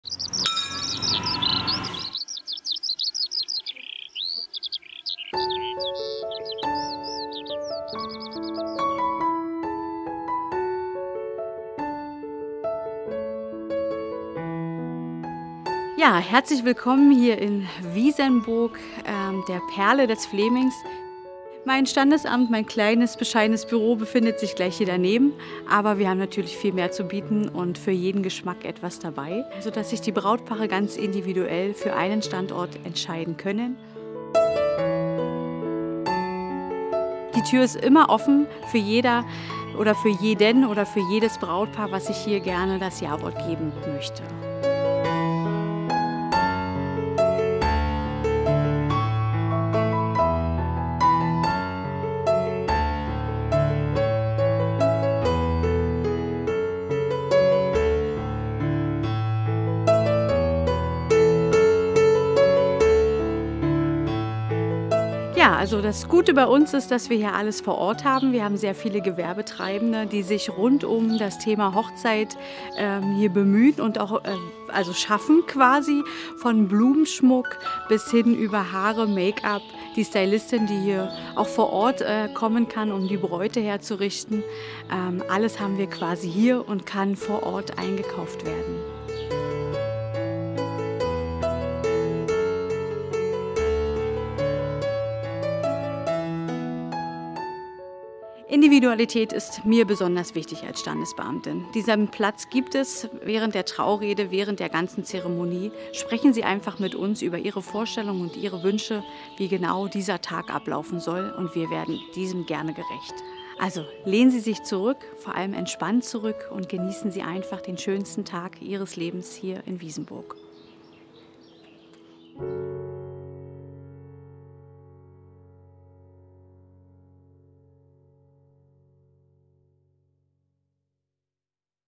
Heiraten in Wiesenburg
Wenn Sie sich eine romantische Location für Ihren großen Tag wünschen, ist der Schlosspark Wiesenburg genau das Richtige für Sie!